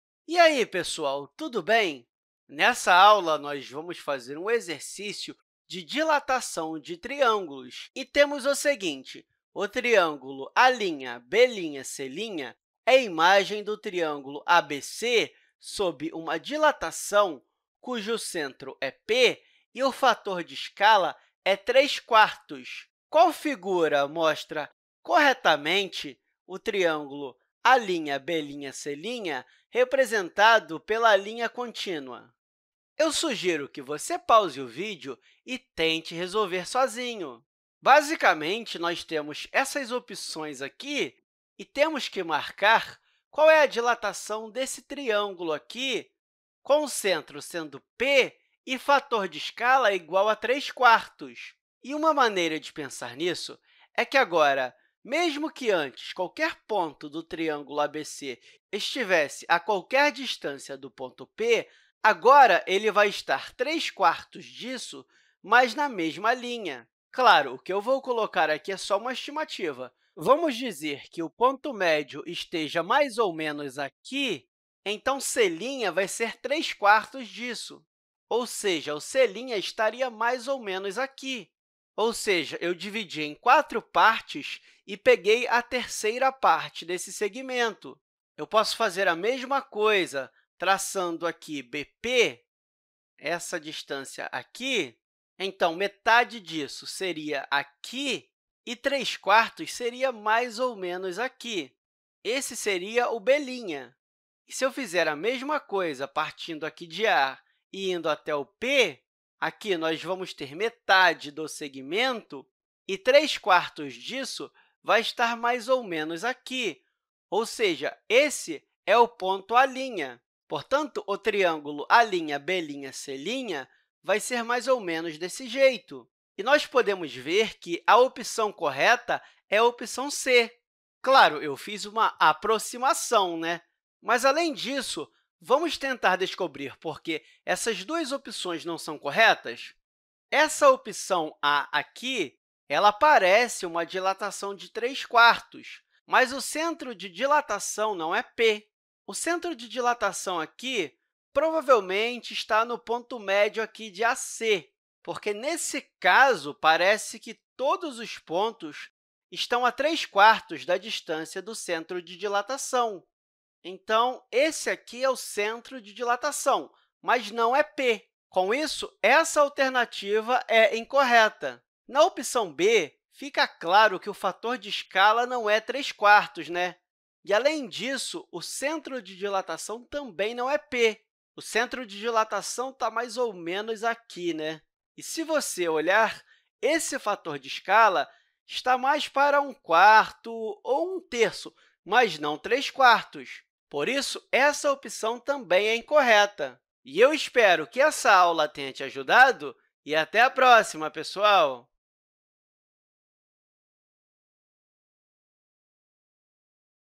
Exercício sobre dilatação de triângulos
Neste vídeo resolvemos um exercício sobre dilatação de triângulos com centro e fator de escala.